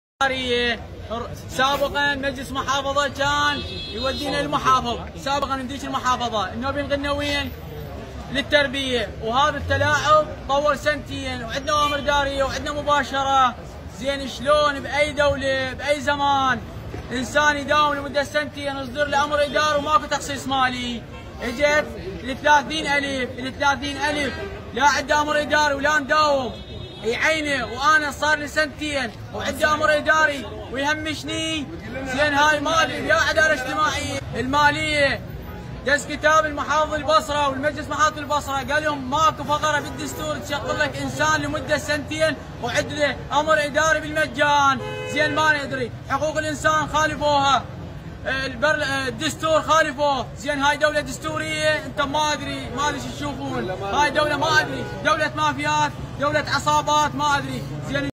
متظاهرو البصرة يغلقون ميناء المعقل والبوابة الرئيسية المؤدية إلى مبنى ديوان المحافظة
متظاهرو البصرة يغلقون ميناء المعقل والبوابة الرئيسية المؤدية إلى مبنى ديوان المحافظة احتجاجًا على الفساد الحكومي
ـــــــــــــــــــــ